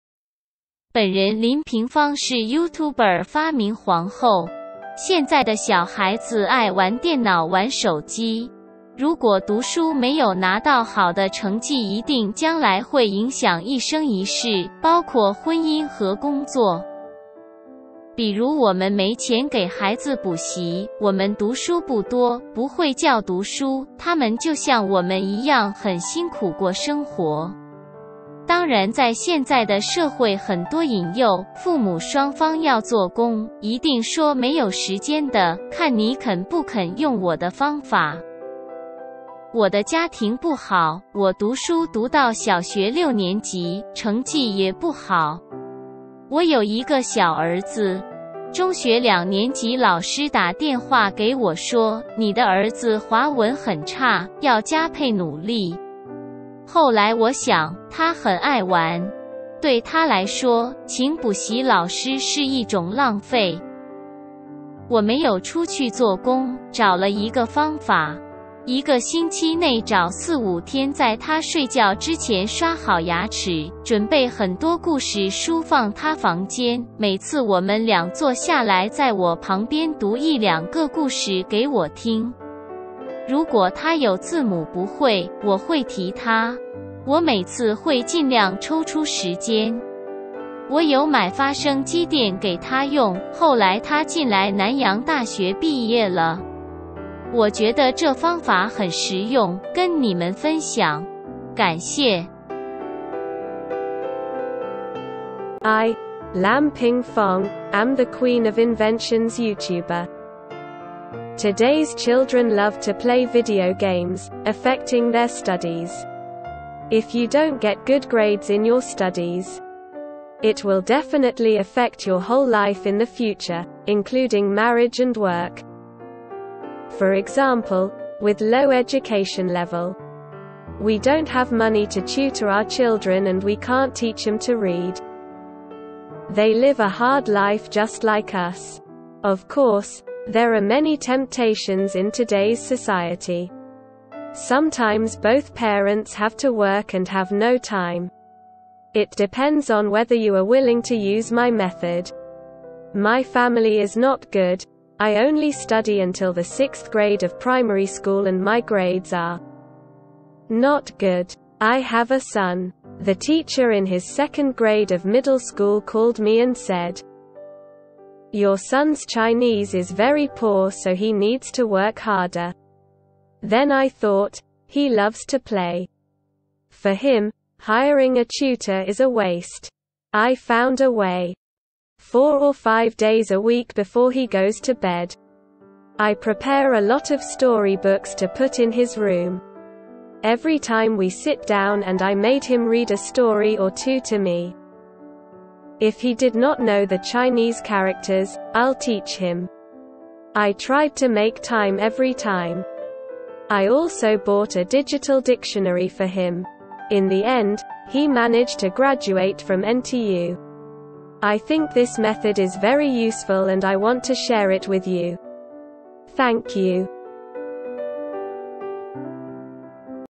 （林平方） 帮助你们孩子读书的好方法 Teach Your KIds Studies
本人林平方是YouTuber发明皇后。现在的小孩子爱玩电脑玩手机。如果读书没有拿到好的成绩一定将来会影响一生一世，包括婚姻和工作。比如我们没钱给孩子补习，我们读书不多，不会教读书，他们就像我们一样很辛苦过生活。当然在现在的社会很多引诱，父母双方要做工，一定说没有时间的，看你肯不肯用我的方法。我的家庭不好，我读书读到小学六年级，成绩也不好。我有一个小儿子。中学两年级老师打电话给我说：你的儿子华文很差，要加配努力。后来我想，他很爱玩。对他来说，请补习老师是一种浪费。我没有出去做工，找了一个方法。一个星期内找四五天在他睡觉之前刷好牙齿，准备很多故事书放他房间，每次我们两坐下来在我旁边读一两个故事给我听。如果他有字母不会，我会提他。我每次会尽量抽出时间。我有买发声字典给他用，后来他进来南洋大学毕业了。我觉得这方法很实用，跟你们分享。感谢。

I, Lam Ping Fong, am the queen of inventions YouTuber. Today's children love to play video games, affecting their studies. If you don't get good grades in your studies, it will definitely affect your whole life in the future, including marriage and work. For example, with low education level, we don’t have money to tutor our children and we can’t teach them to read. They live a hard life just like us. Of course, there are many temptations in today's society. Sometimes both parents have to work and have no time. It depends on whether you are willing to use my method. My family is not good, I only study until the sixth grade of primary school and my grades are not good. I have a son. The teacher in his second grade of middle school called me and said: Your son's chinese is very poor so he needs to work harder. Then I thought, he loves to play. For him, hiring a tutor is a waste. I found a way. Four or five days a week before he goes to bed, I prepare a lot of storybooks to put in his room. Every time we sit down and I made him read a story or two to me. If he did not know the Chinese characters, I'll teach him. I tried to make time every time. I also bought a digital dictionary for him. In the end, he managed to graduate from NTU. I think this method is very useful and I want to share it with you. Thank you.